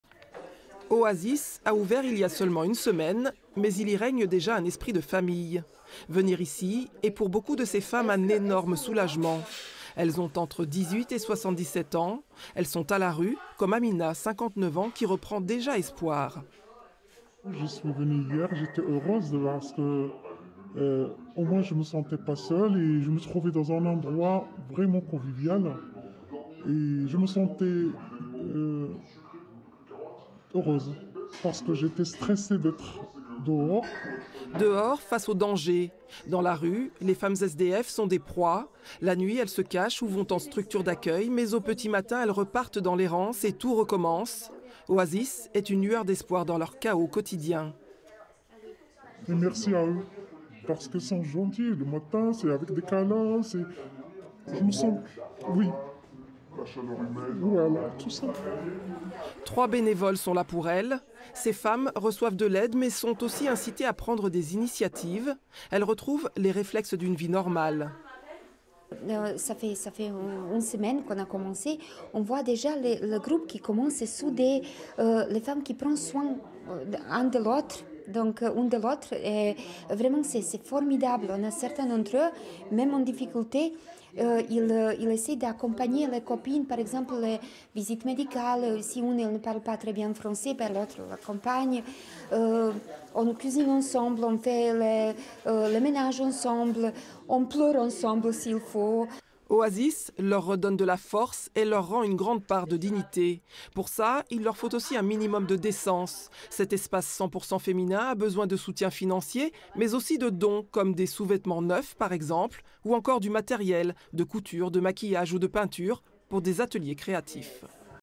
Nice : ouverture d'un centre d'accueil de jour pour les femmes sans abris
Les accueils de jour pour femmes sans abris sont des lieux rares : il n'y en a que 6 en France.
A Nice, l'un de ces espaces vient d'ouvrir. Baptisé OASIS, cette structure est devenue en quelques jours un véritable cocon pour des femmes exposées quotidiennement aux dangers de la rue.